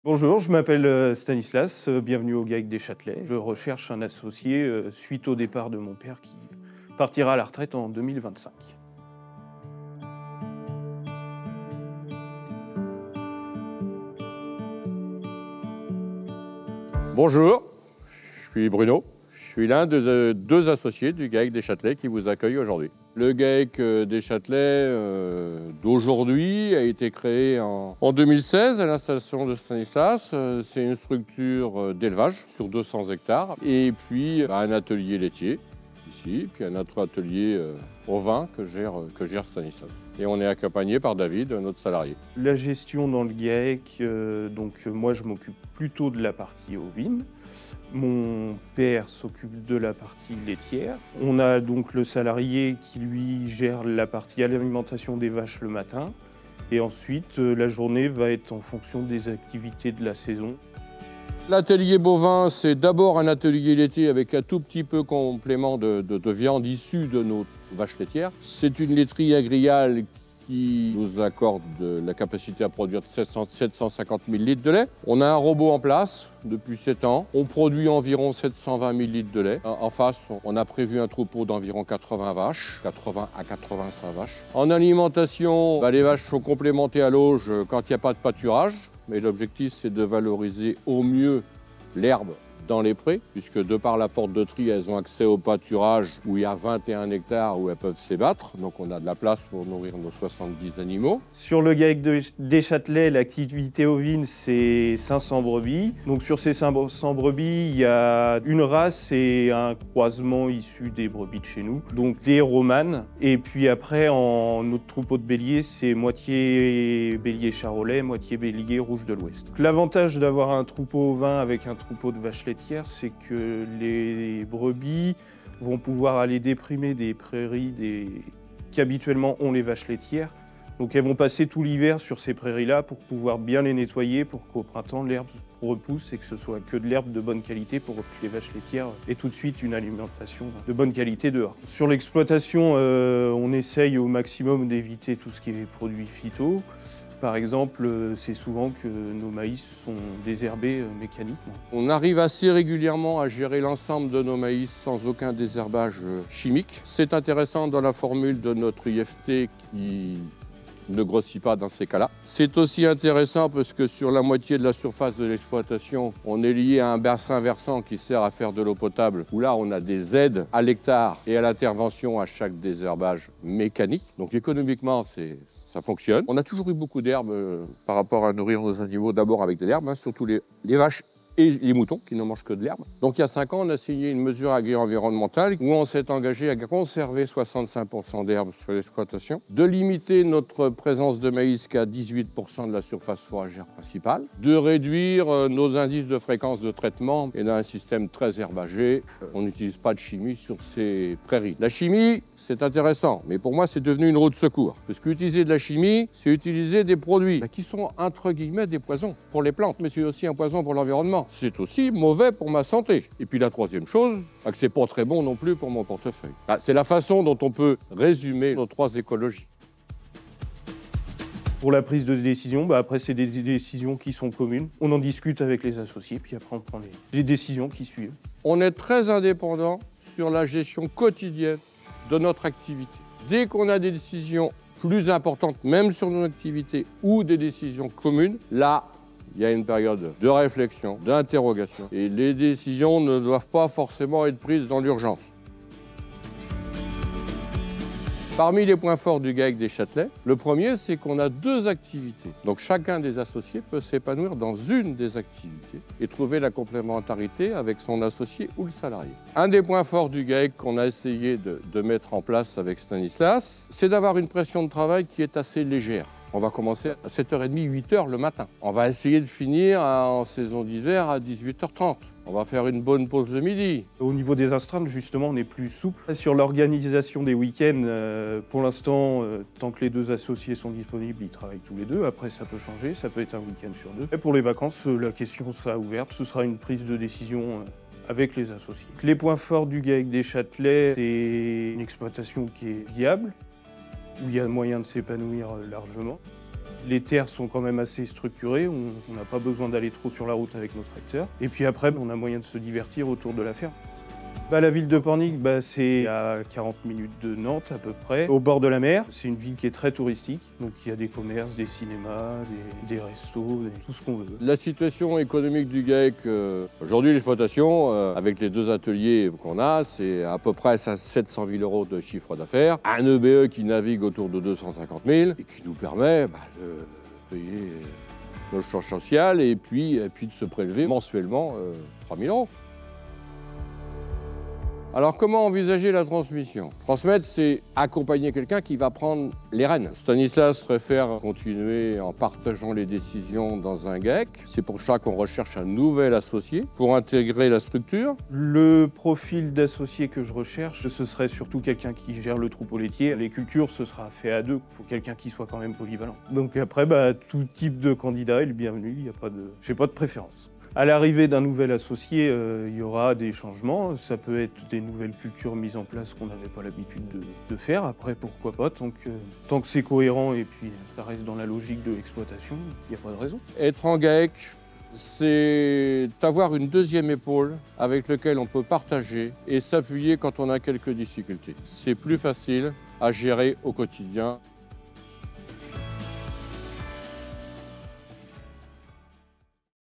Découvrez le GAEC des Châtelets à Pornic, en Loire-Atlantique (44)
☎️ Pour plus d'informations, rendez-vous sur le formulaire de prise de contact

[𝐅𝐄𝐑𝐌𝐄 𝐃𝐈𝐒𝐏𝐎𝐍𝐈𝐁𝐋𝐄 - 𝐏𝐎𝐑𝐍𝐈𝐂 (𝟒𝟒)]

🐄 Envie de vous installer en bovin et ovin en système herbager ?

📍Installez-vous à Pornic, en Loire-Atlantique, à 4km de l'océan Atlantique et 50 minutes de Nantes.

Rejoignez le GAEC des Châtelets !
Association courant 2025 après une période de stage ou de salariat

Activité à reprendre :
🐄 Élevage de 85 vaches laitières Prim'Holstein, traite robotisée

Autres activités de la ferme :
🐑 Élevage ovin viande
🐂 Petit élevage bovin viande, croisés issus du troupeau laitier

🏘️ Bâtiments agricoles :
élevage bovin
- 1 stabulation 72 places, avec robot de traite (2016)
- 1 bâtiment ovin/veaux
- 1 bâtiment génisses reproduction
- 1 bâtiment d'engraissement taurillons et génisses taries
- silos + fumière + fosse
élevage ovin
- 1 bergerie maternité/mise bas/sevrage
- 1 bergerie engraissement agneaux
- 1 bâtiment tonte/stockage
- silos + fumière

🌾 Foncier :
195 ha de SAU, dont 35 ha drainés et 70 ha en vallons boisés :
- 25 ha de maïs
- 30 ha de céréales
- 5 ha de sorgho
- 20 ha de prairies naturelles
- 120 ha de prairies

🚜 Matériel agricole : liste sur demande

➡️ 📣 Pourquoi s'installer au GAEC des Châtelets ?
- Une réflexion poussée autour des pratiques agroécologiques
- Une situation économique confortable
- Deux activités d'élevages complémentaires
- Un confort de vie facilité par les plages horaires de travail, une ambiance de travail sereine et une localisation géographique agréable

🏷️ Prix d’installation : 100 000€ (en cours de redéfinition), pour 50% des parts sociales du GAEC (une partie des bâtiments agricoles, cheptel, matériel, stocks et avances aux cultures)